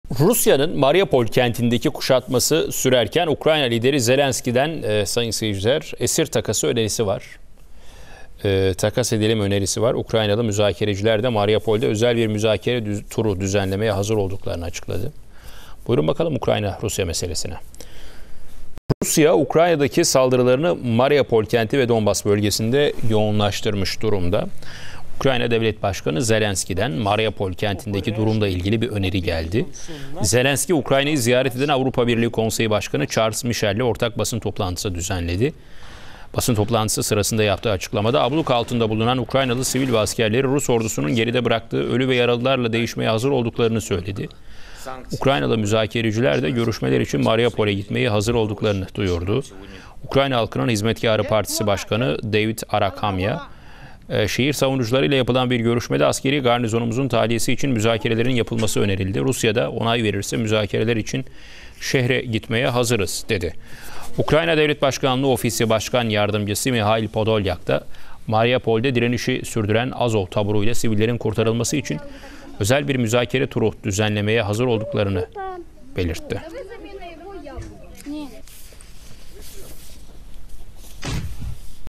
Zelenski'den Rusya'ya Mariupol Teklifi: Hazırız!
Ukrayna Devlet Başkanı Vladimir Zelenski, Mariupol'de ablukada bulunan Ukraynalı sivil ve askerleri, Rus ordusunun geride bıraktığı ölü ve yaralılarla değişmeğe hazır olduklarını söyledi.